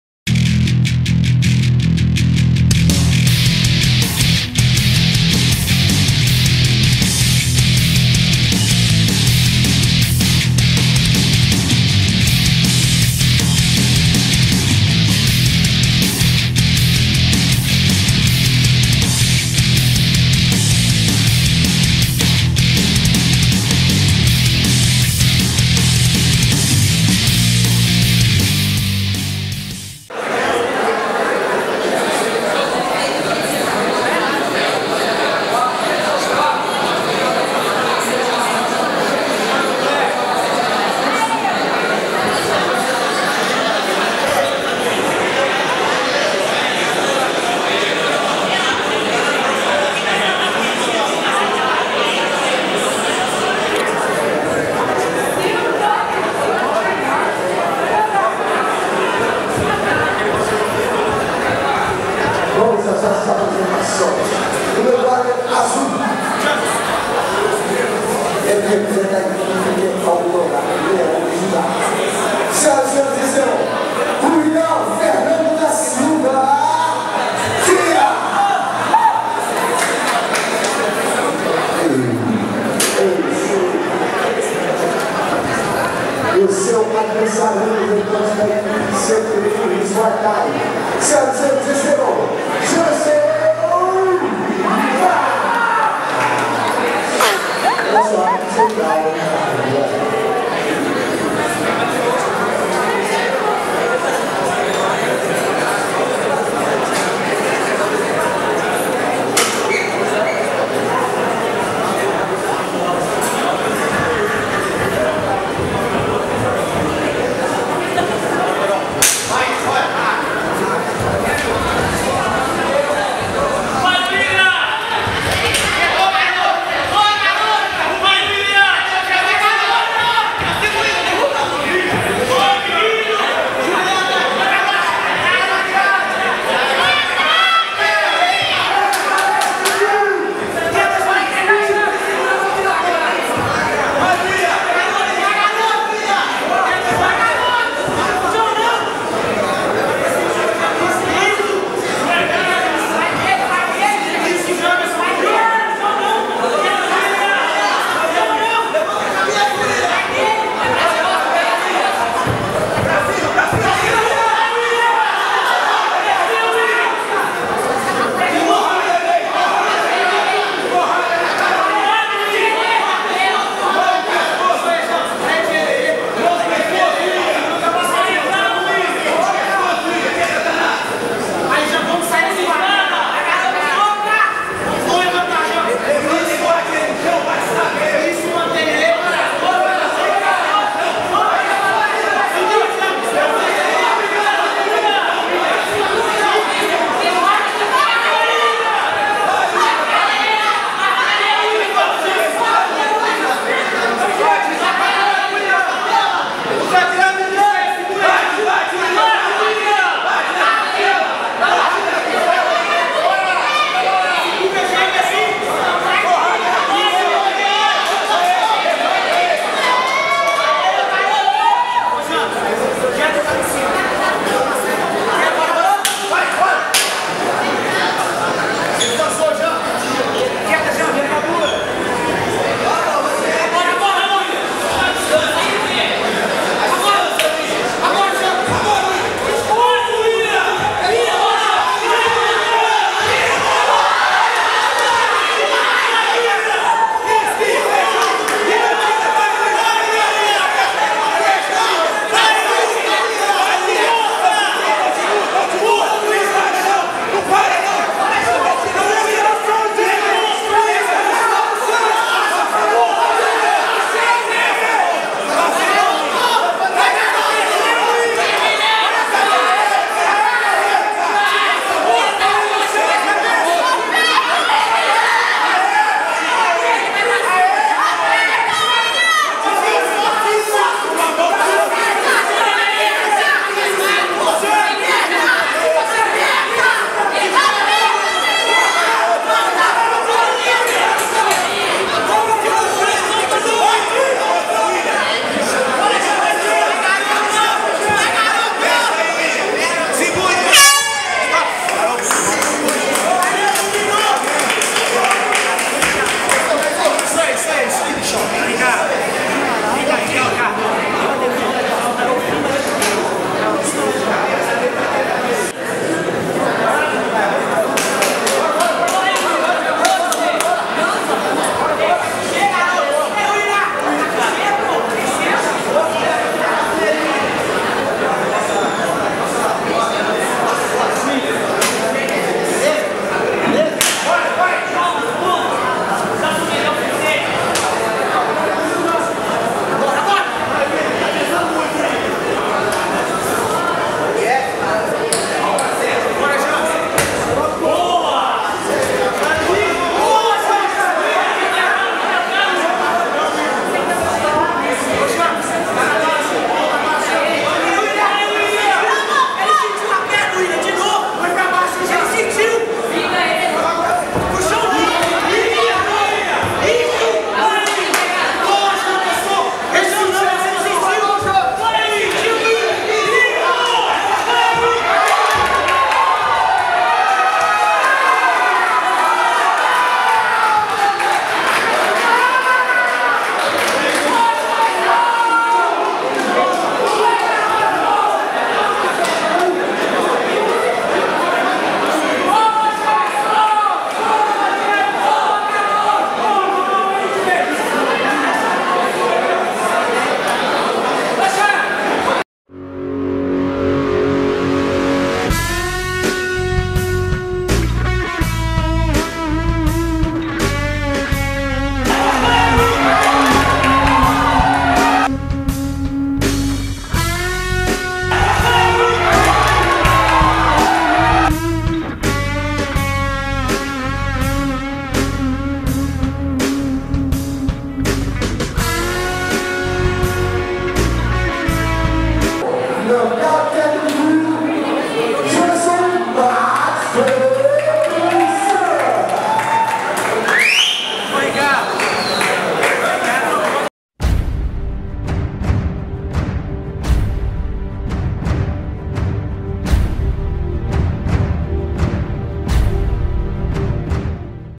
Jamerson vs Willian - BH Sparta 9
Jamerson Max (CT Luiz Muay Thai) venceu William Fernando da Silva “Paraíba” (Leão de Judá) por nocaute aos 0:42 do segundo round.